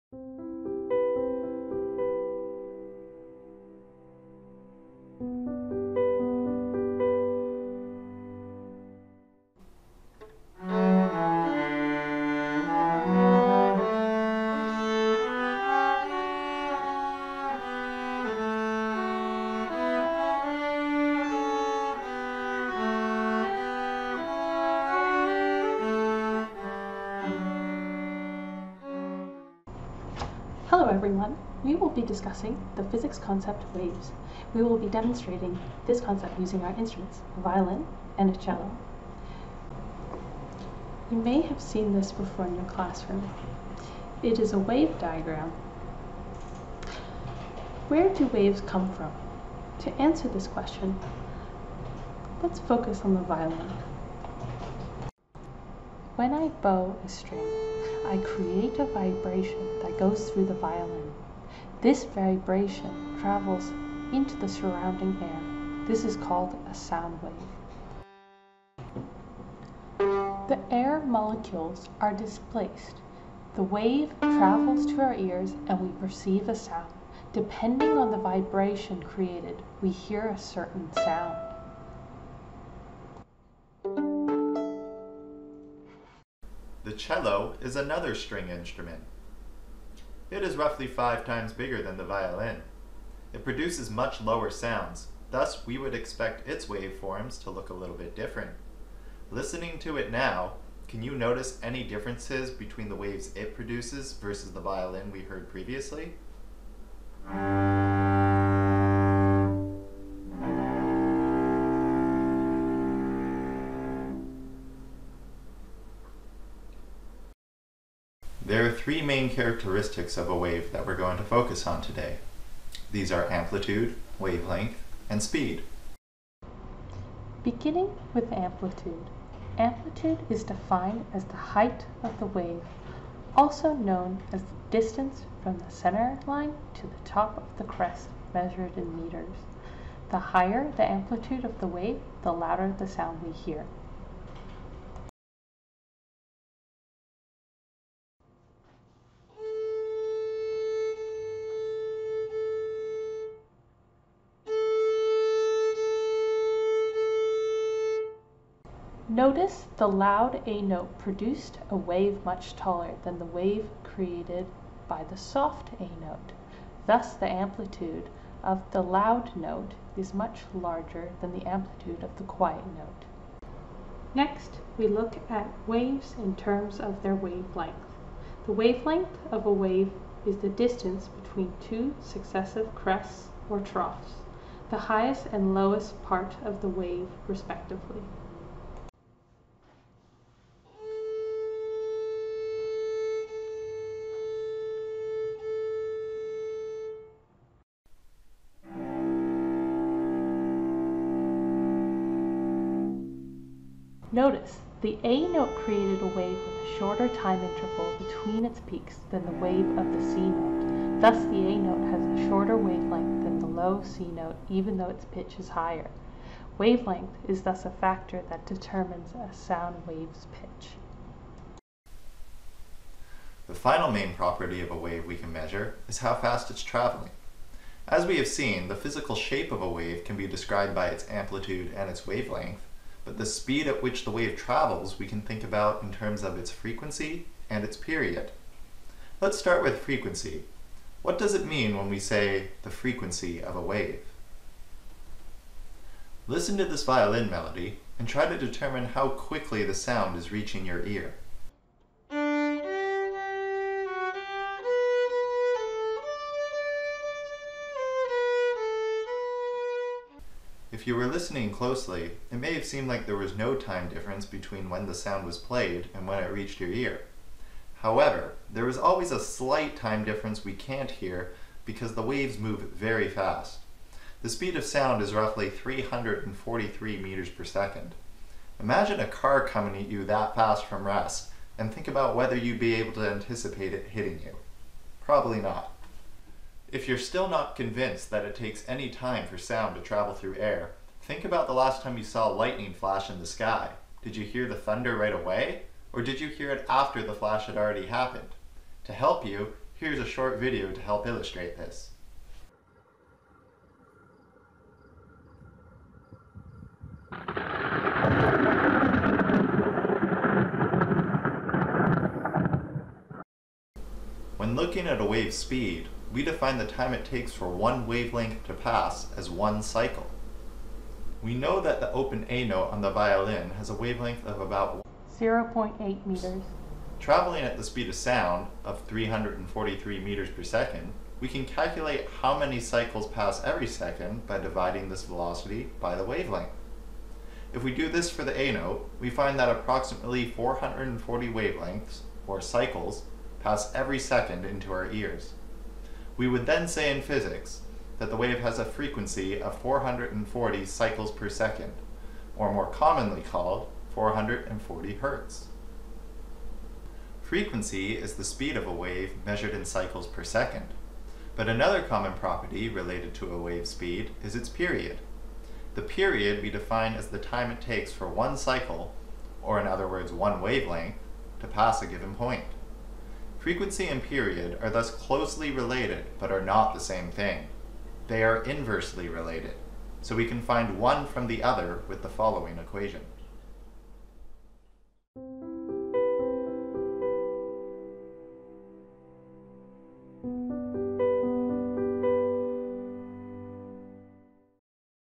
The Physics of Music: Exploring the physics of string instruments.
This video created by UBC future physics teachers will help you understand how string instruments create sound and how we can describe the properties of sound waves. It will also give you ideas about how you can do these experiments yourself at home or in the classroom. Enjoy!